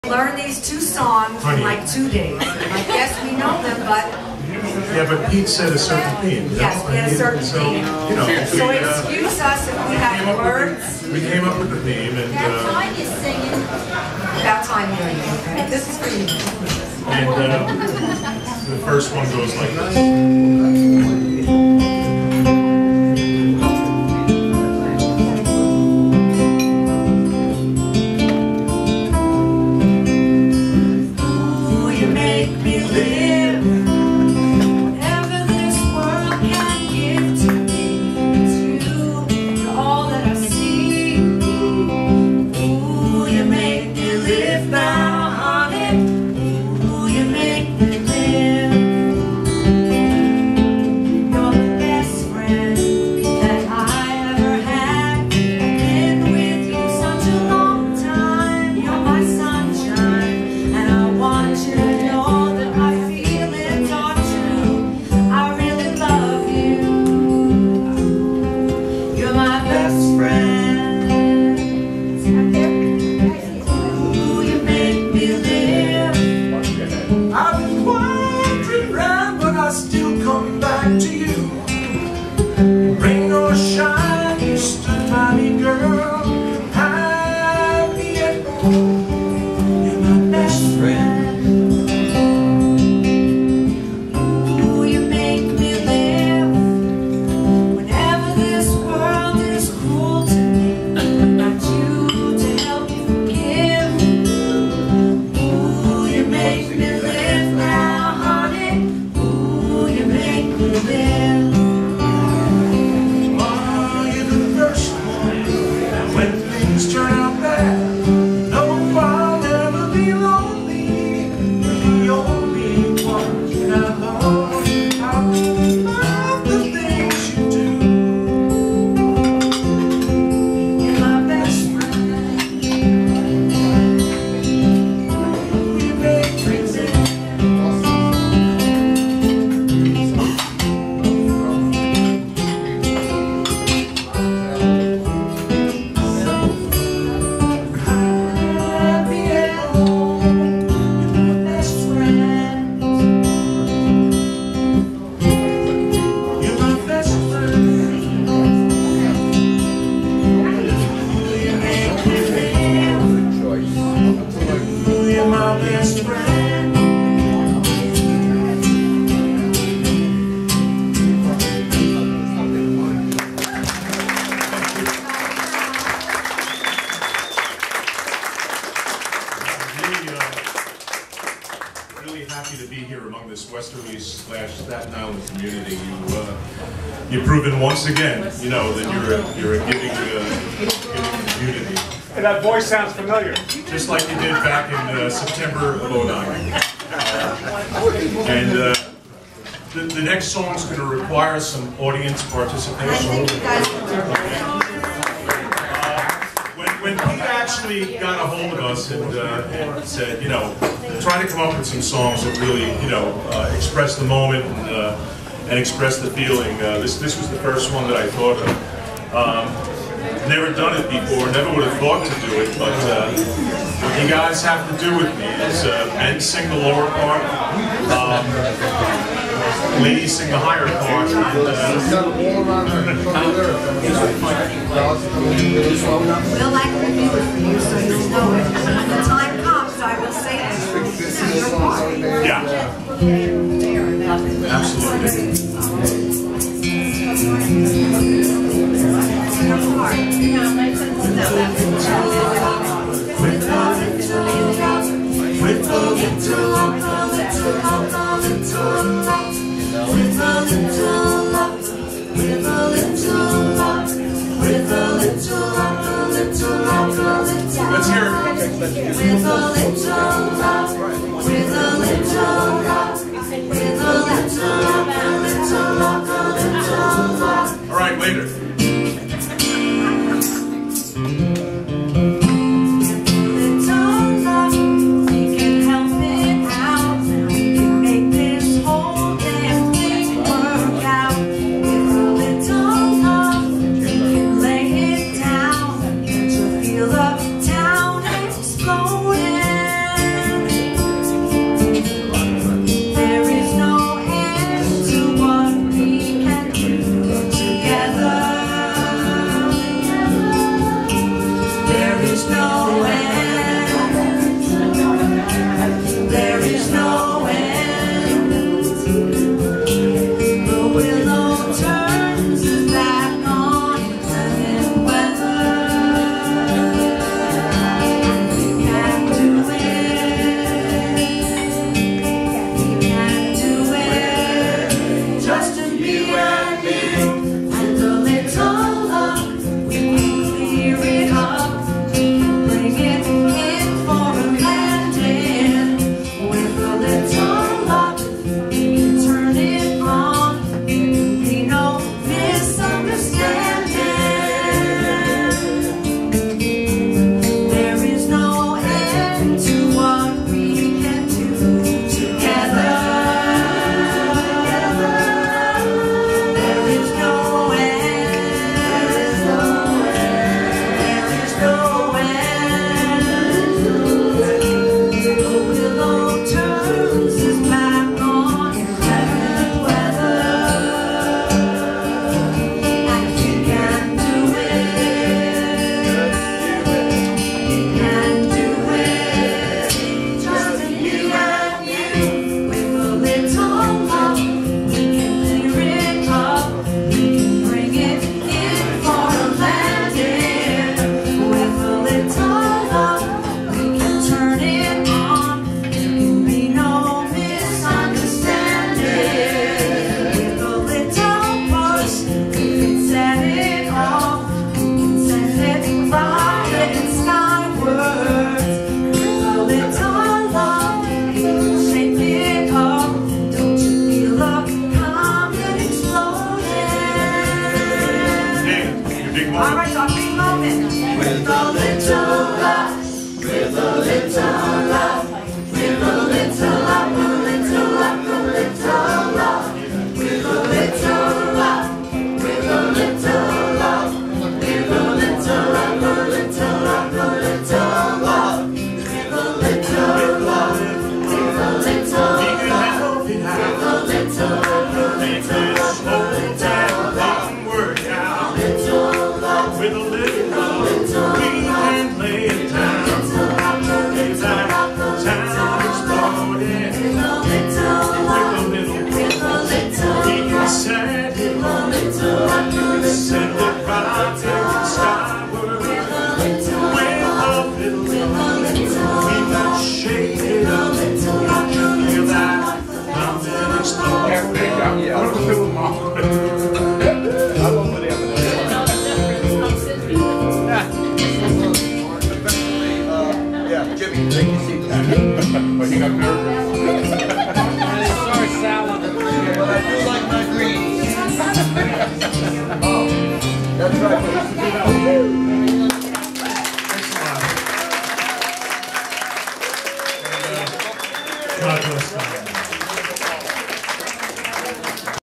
Tom and sue
Tom and Sue singing at Valery Fundrasing at Jimmy Max Resturant.